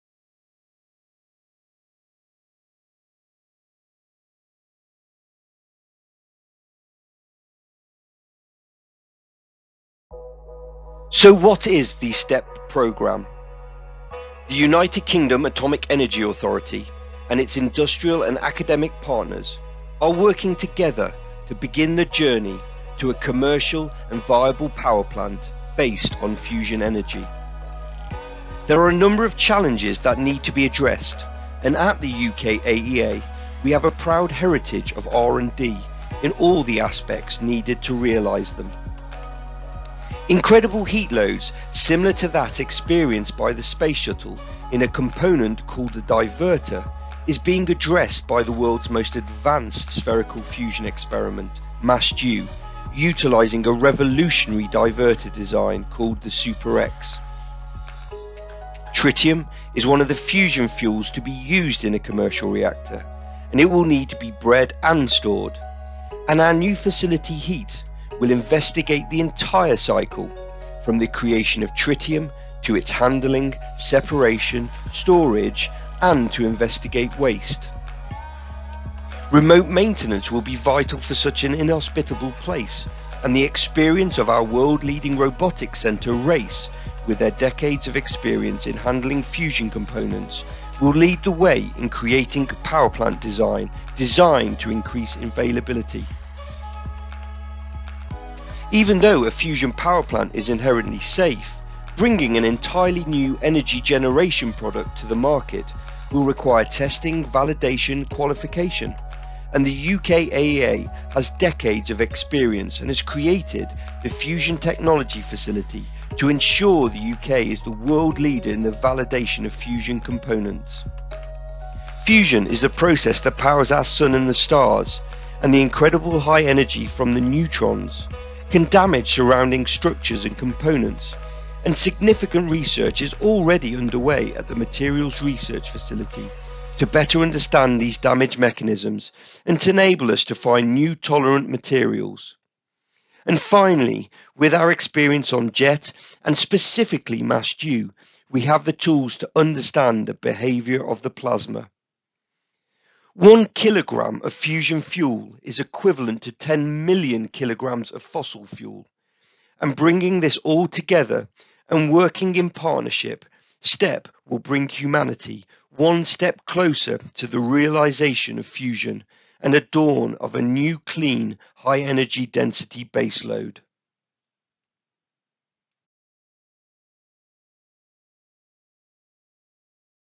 A guide to STEP; UKAEA’s compact fusion reactor programme
Want to find out what goes on inside a fusion energy reactor? This short video gives an insight into the concept design of UKAEA’s Spherical Tokamak for Energy Production (STEP) reactor. The machine is scheduled to be built by 2040, producing electricity from fusion that can be used on the national grid, which could be a world first!

The STEP programme will develop and identify solutions to the challenges of delivering fusion energy, benefiting from Culham’s breadth of expertise and its suite of research facilities – RACE, Materials Research Facility, H3AT and Fusion Technology Facilities.